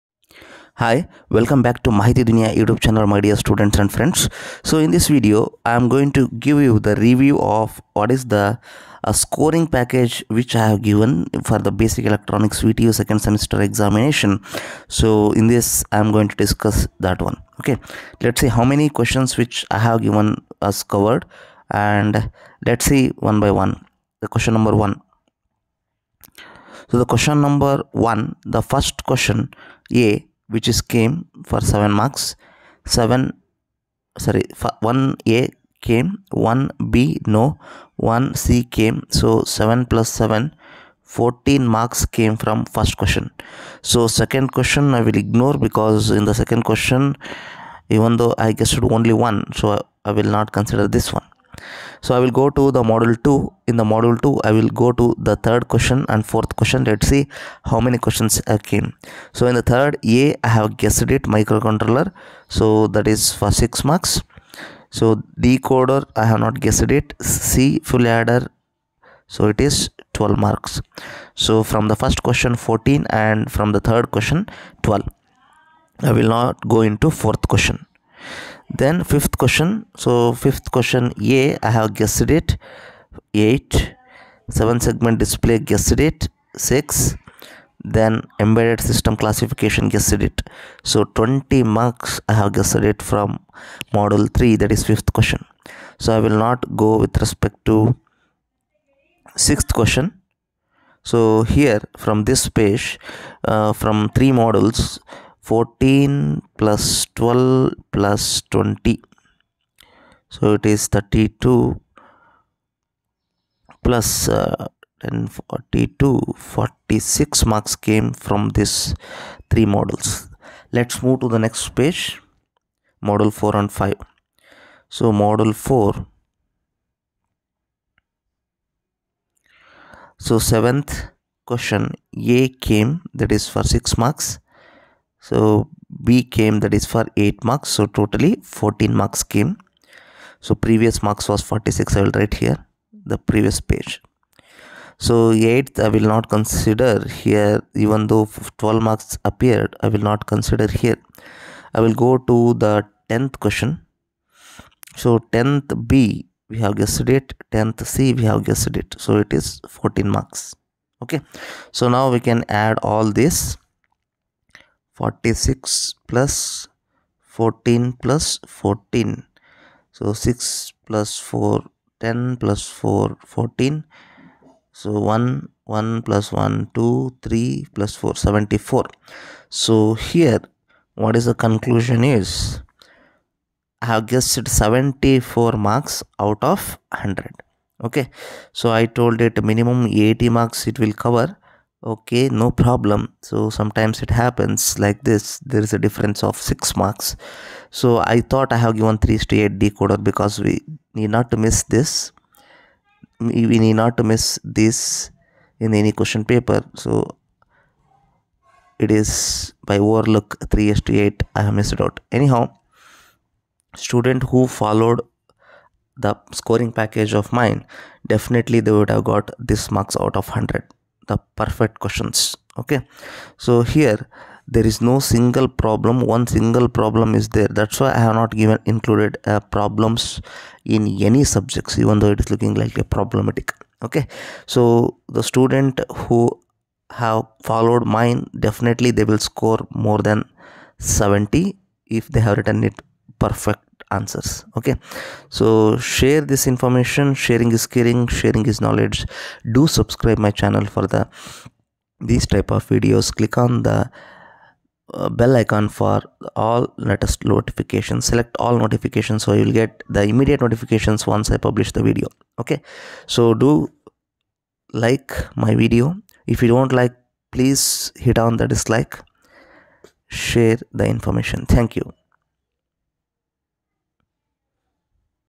VTU 2nd Sem |Basic Electronics Qp Review with scoring package|74 out of 100 appeared in QP.
mechanical passing package